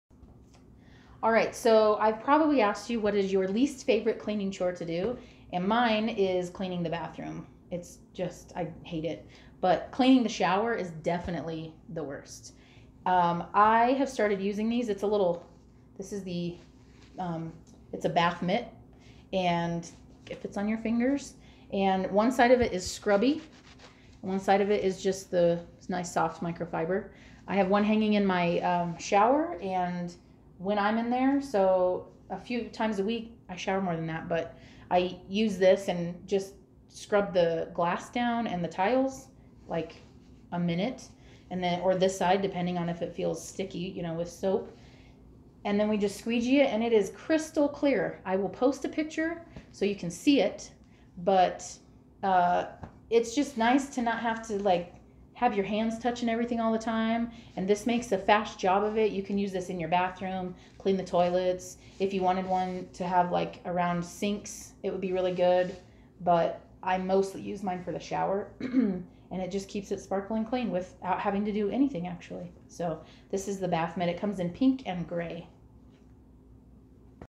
Norwex bathroom scrub mitt
Microfiber on one side and scrub mesh on the other combine to easily remove stubborn dirt and grime.  I use this to keep my shower crystal clear without hardly any elbow grease and zero chemicals!